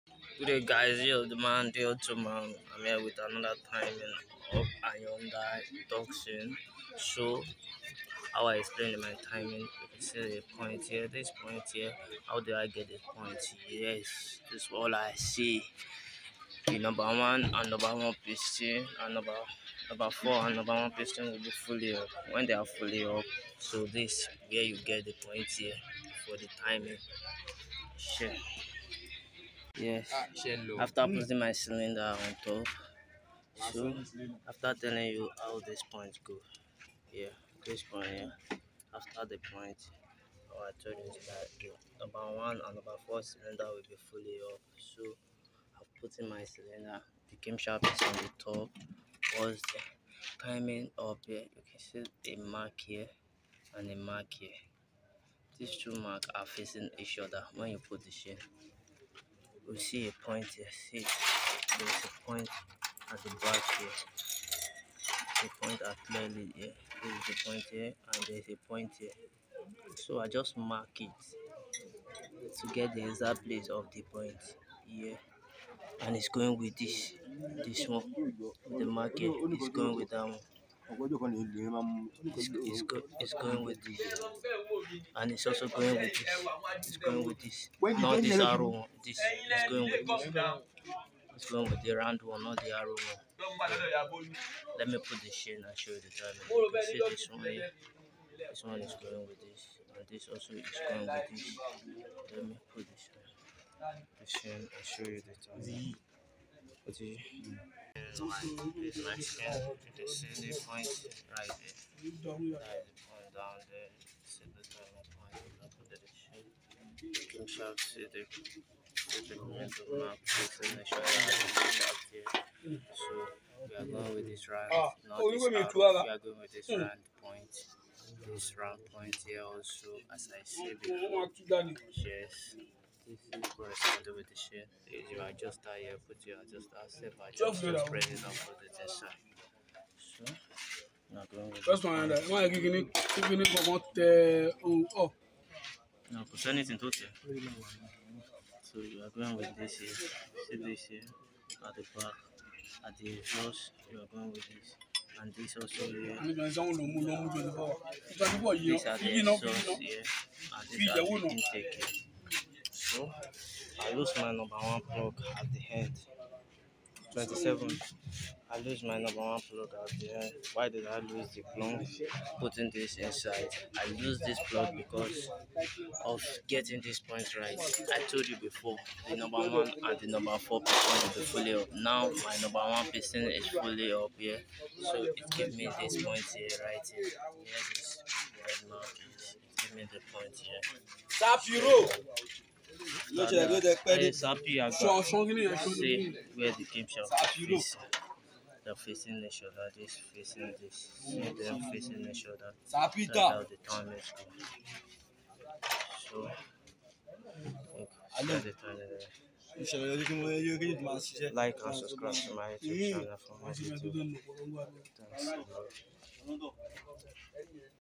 Hyundai G4NC Engine timing chain installation
The video is all about how to set the timing of hyundai tucson G4NC engine timing chain ⛓️ setting with explanation